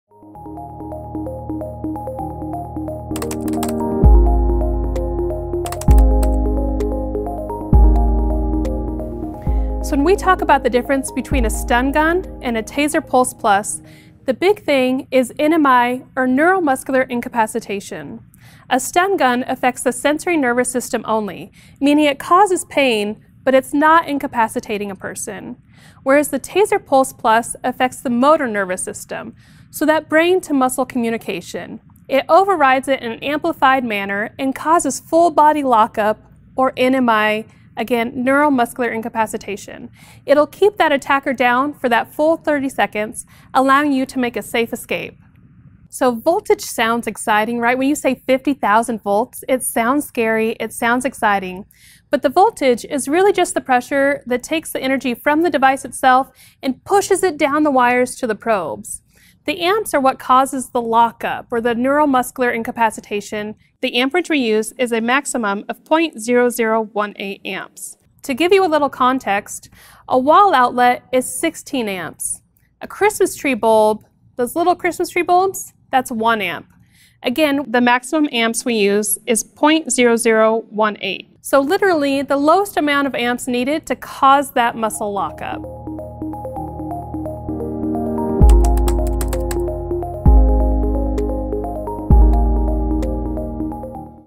Pulse+ Training: What is NMI?
TASER Senior Trainer Sara explains Neuro Muscular Incapacitation (NMI).